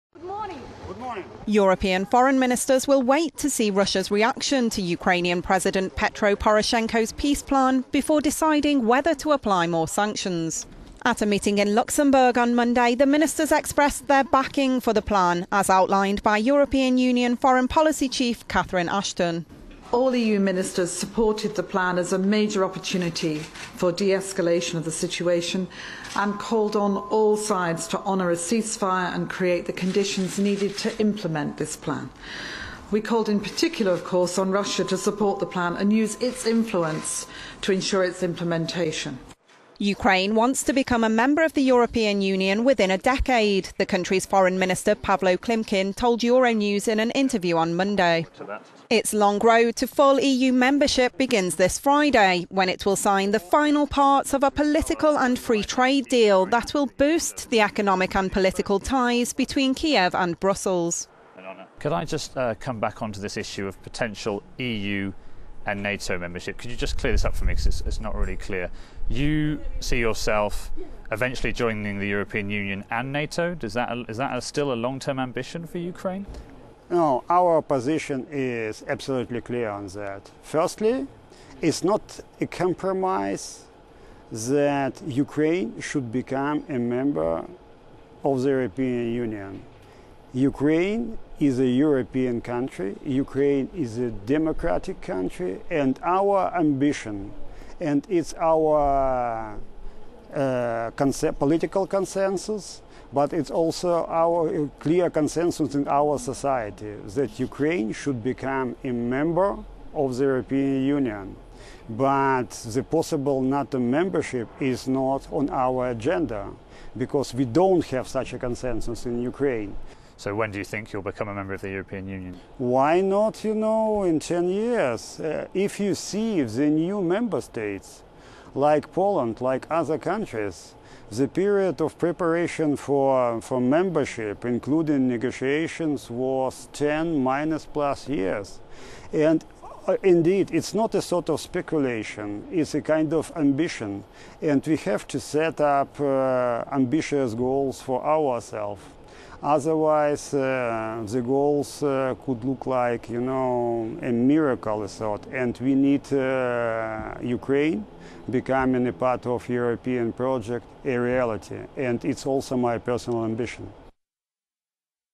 Ukraine Foreign Minister sees EU membership within 10 years
European foreign ministers will wait to see Russia's reaction to Ukrainian President Petro Poroshenko's peace plan before deciding whether to apply more sanctions.

At a meeting in Luxembourg on Monday, the ministers expressed their backing for the plan, as European Union Foreign Policy Chief Catherine Ashton outlined.

"All the EU ministers supported the plan as a major opportunity for de-escalation of the situation and called on all sides to honour a cease fire and create the conditions to im...